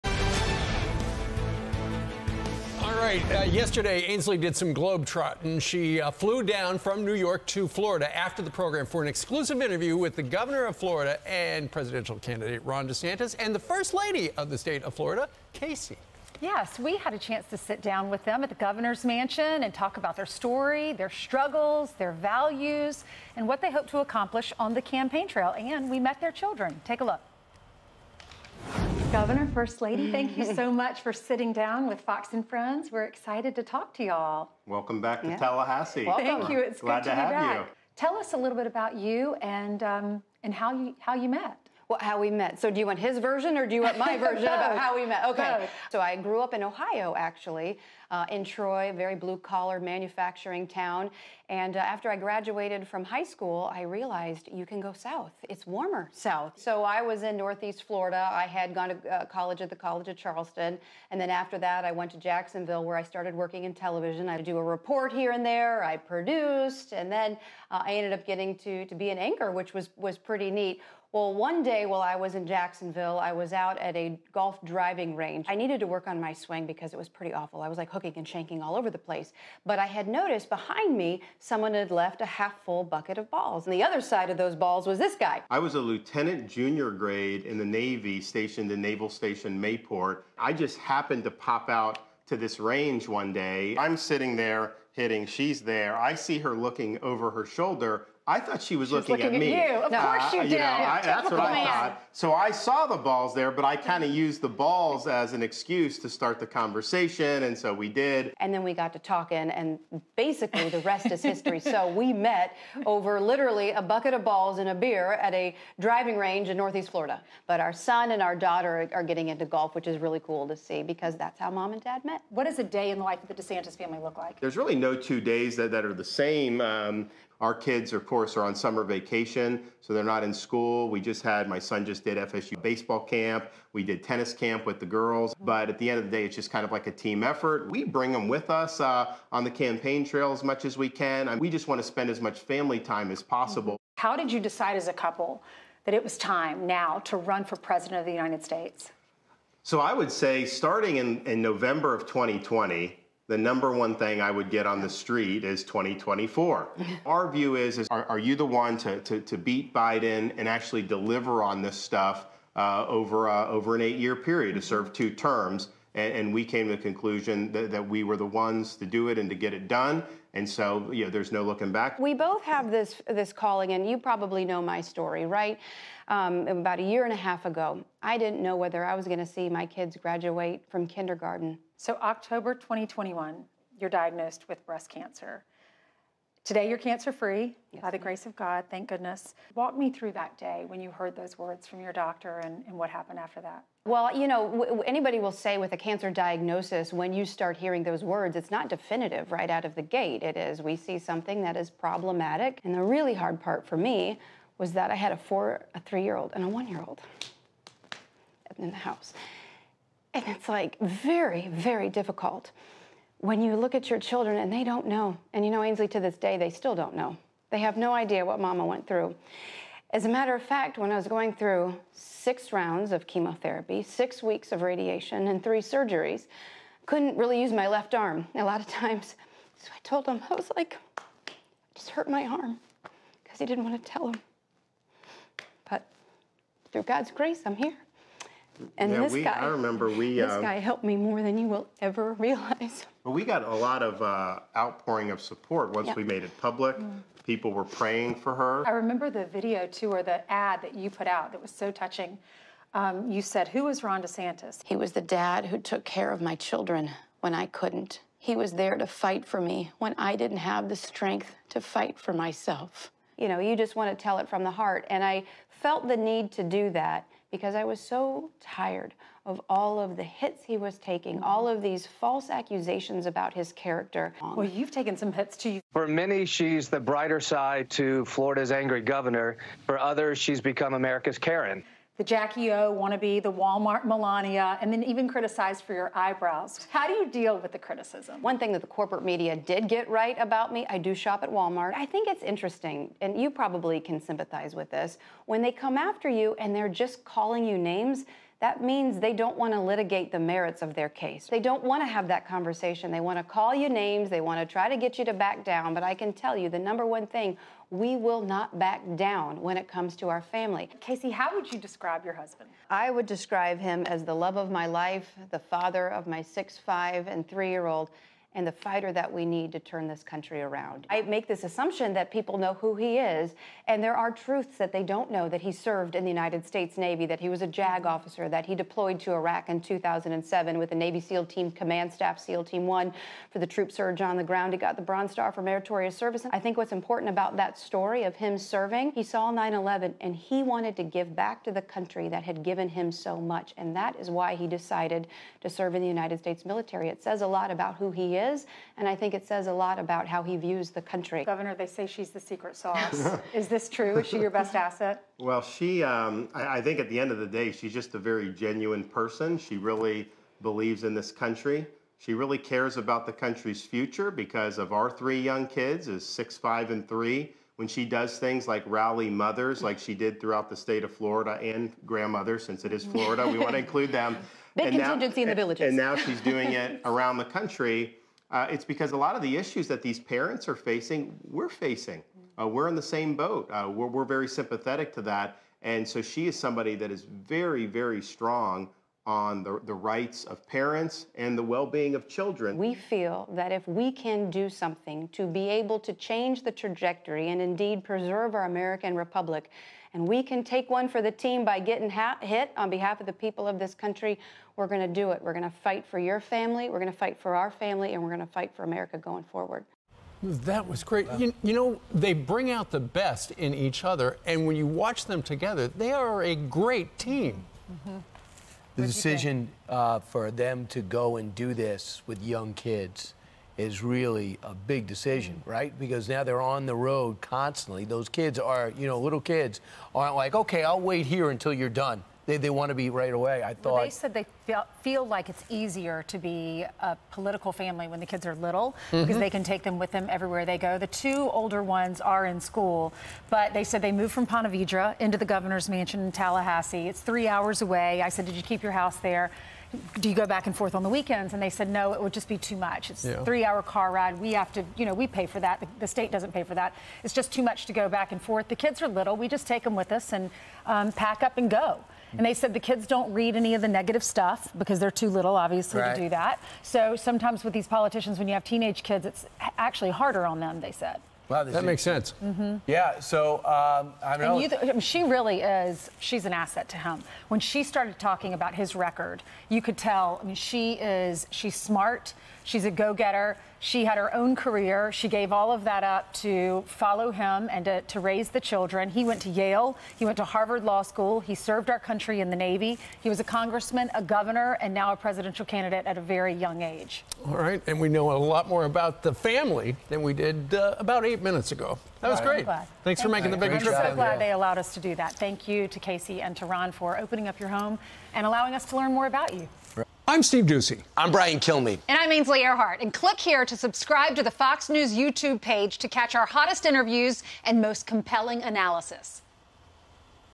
Casey DeSantis hits back at 'Walmart Melania' criticism: Will not back down'
Gov. Ron DeSantis, R-Fla., and his wife Casey spoke with 'Fox & Friends' co-host Ainsley Earhardt for an exclusive sit-down interview.